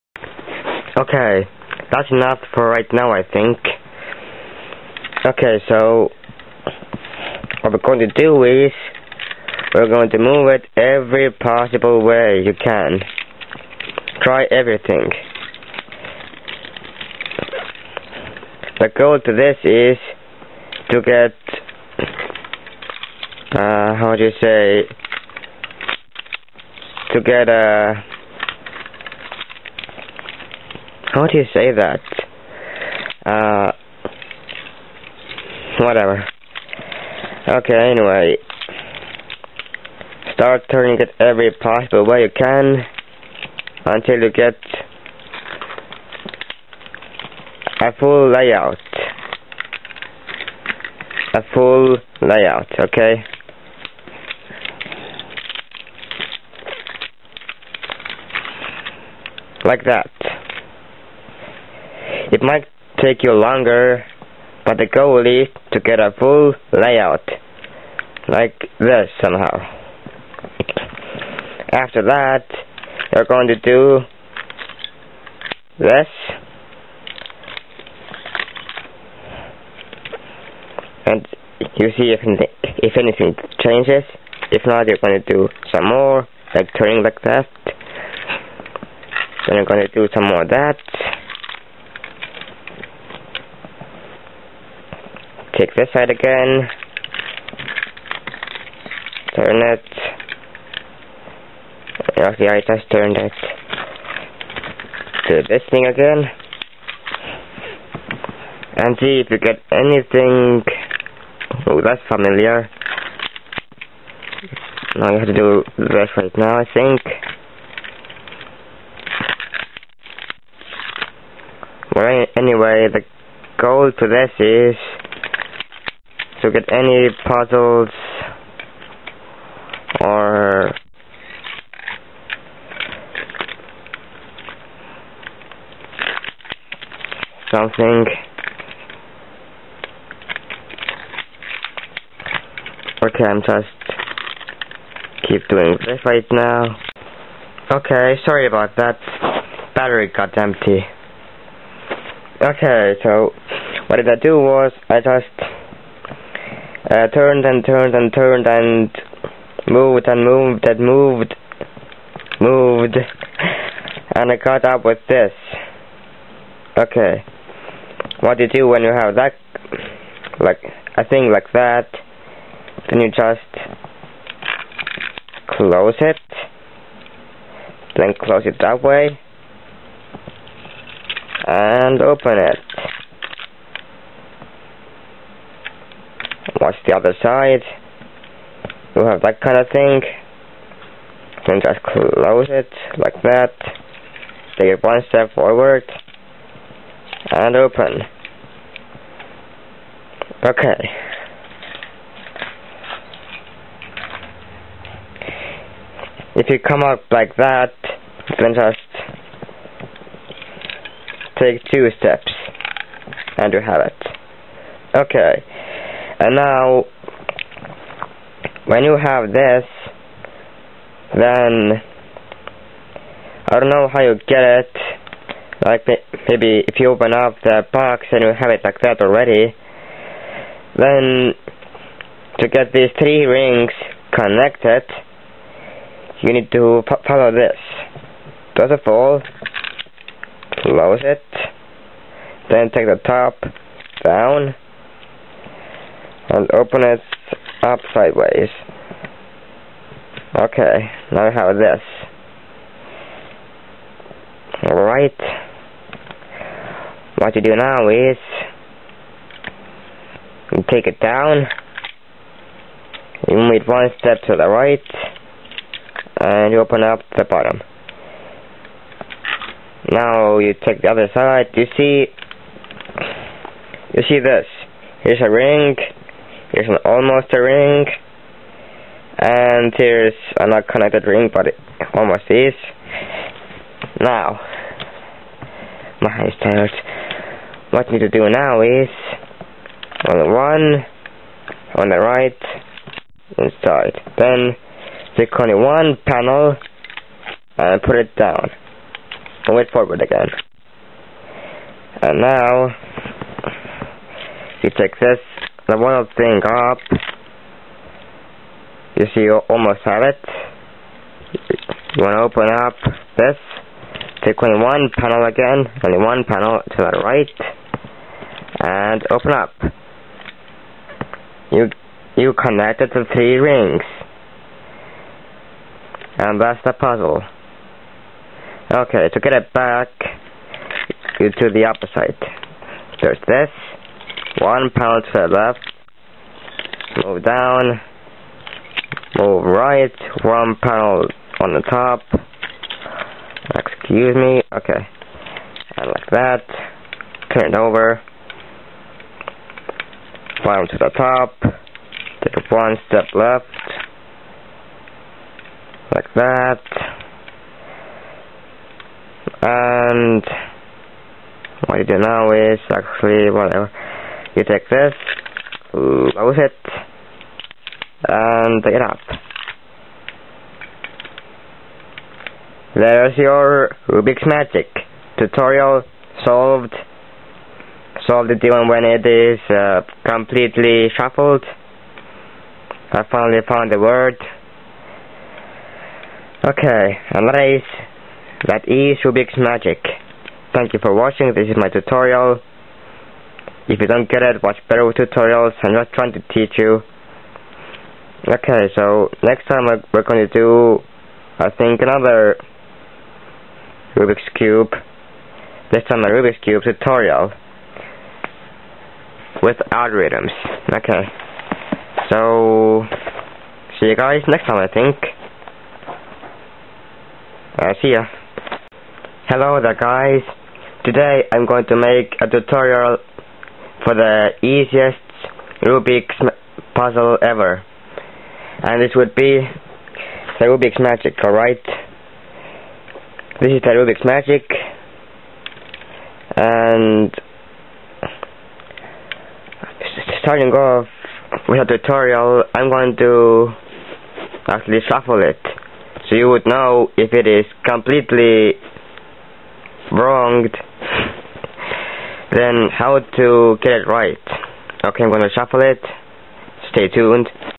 (Old Video) Rubiks Magic Tutorial
A real life video (with bad music?) that was made a long time ago that I decided to re-upload on a different channel for potential copyright claim prevention due to joining Maker Studios